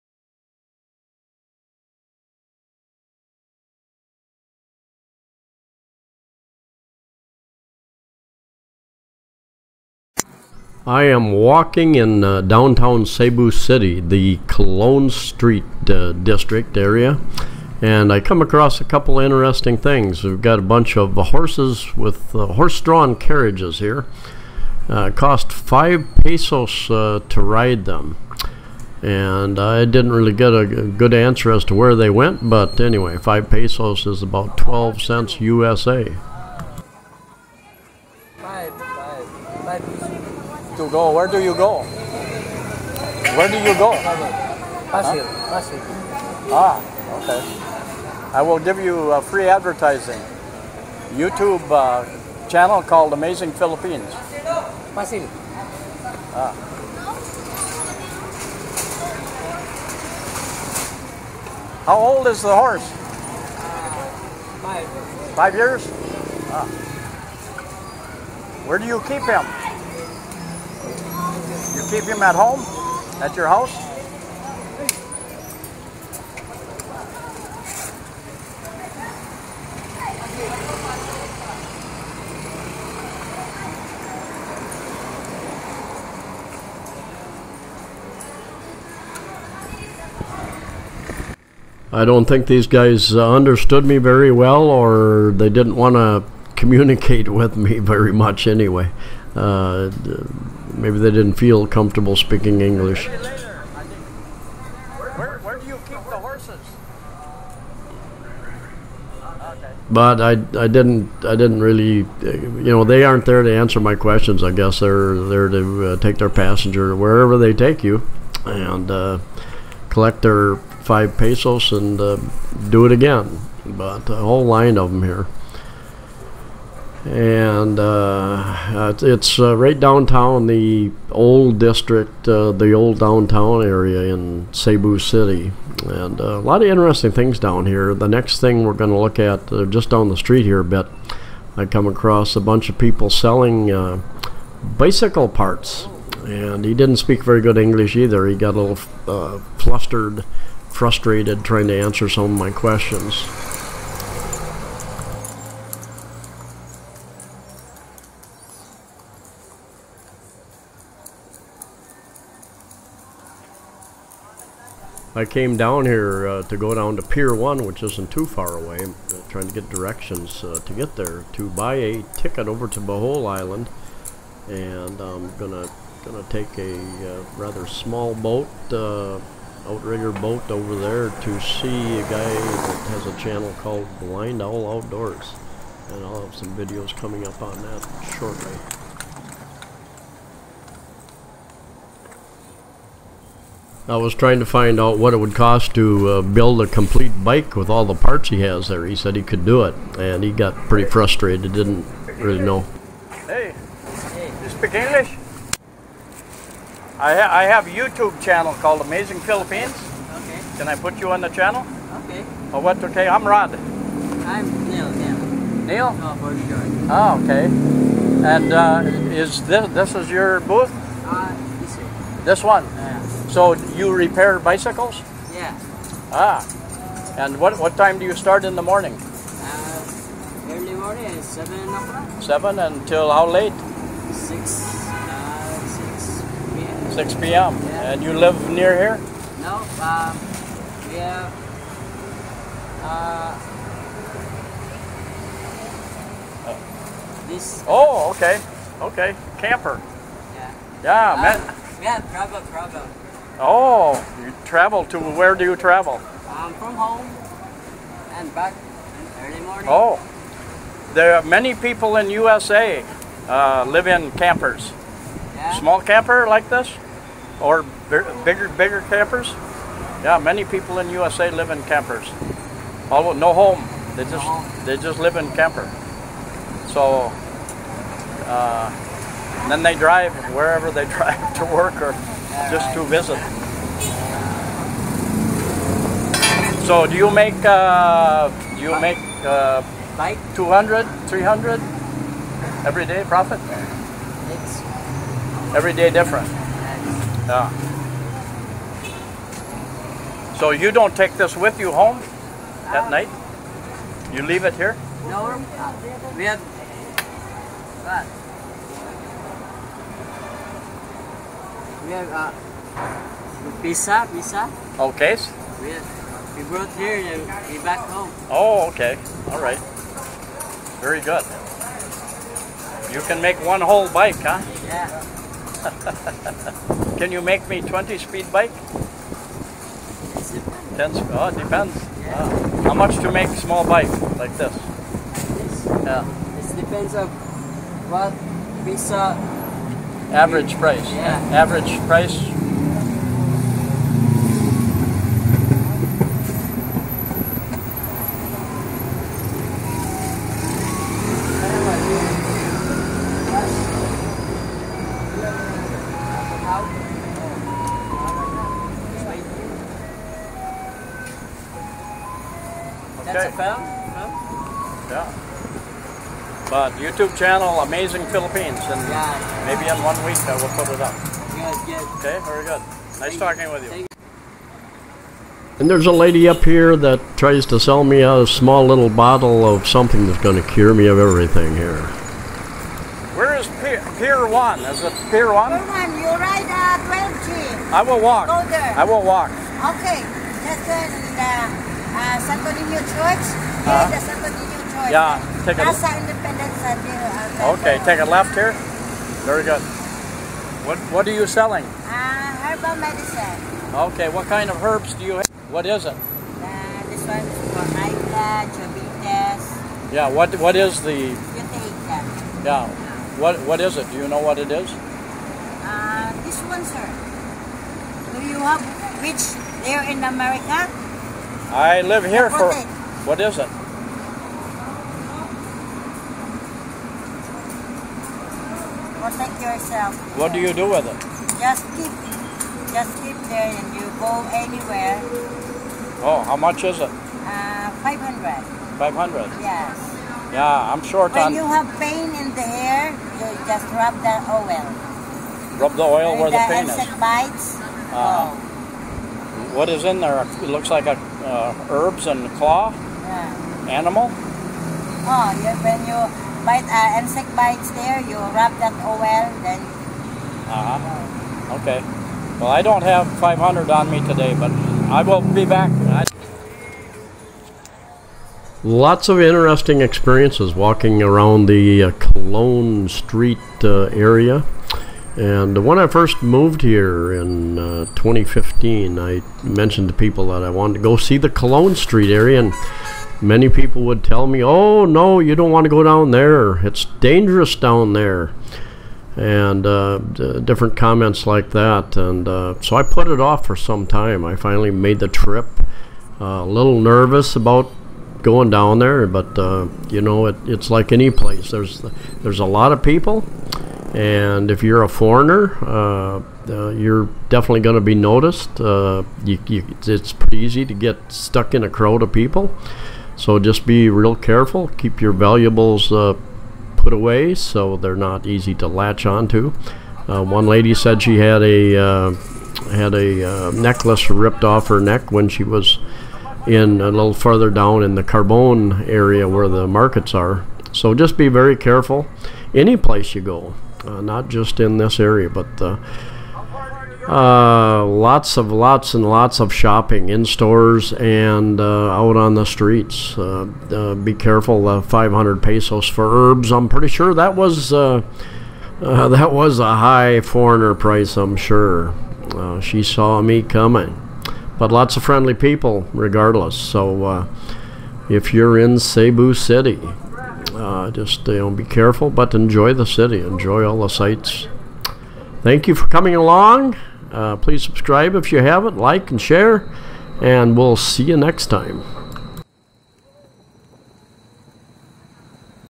WALKING ENCOUNTERS ON COLON STREET
Lots of interesting experiences walking the Cebu City downtown area around Colon Street.  Horse buggy rides, bicycle parts and herb lady are just some of them.